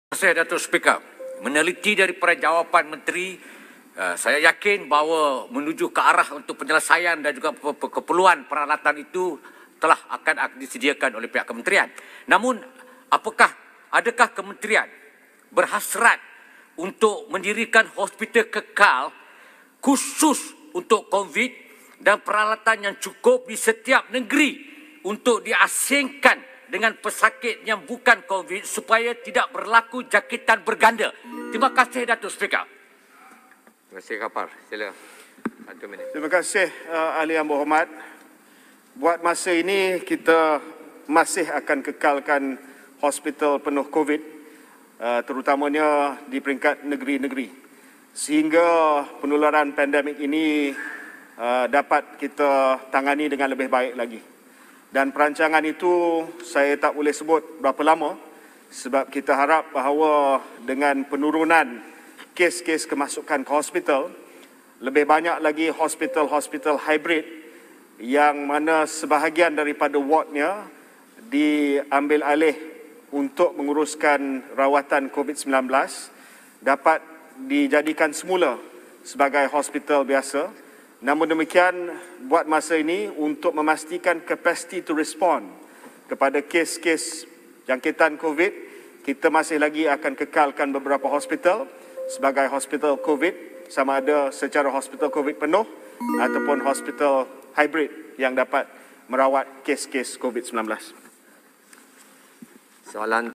KKM bercadang kekalkan hospital penuh COVID-19 di peringkat negeri
Menteri Kesihatan, Khairy Jamaluddin memberitahu Kementerian Kesihatan (KKM) akan kekalkan hospital penuh COVID-19 di peringkat negeri sehingga penularan dapat dikawal sepenuhnya.